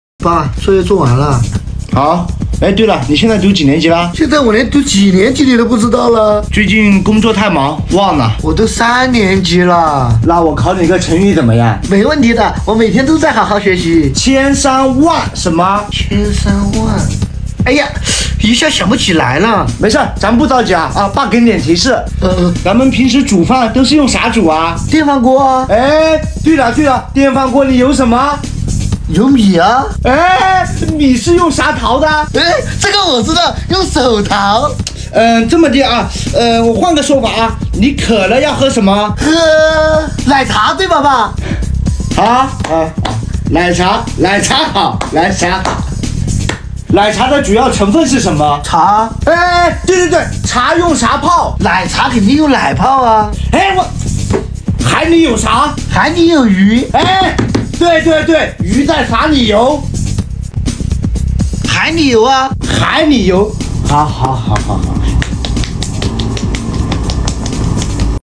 辅导孩子作业怎么这么难？一个组词都想不到，孩子到底随了谁？【小德子】
辅导孩子作业怎么这么难？一个组词都想不到，孩子到底随了谁？
好看的皮囊千篇一律，有趣的灵魂万里挑一
这是一个有趣的频道。
每天给大家分享有趣优质的视频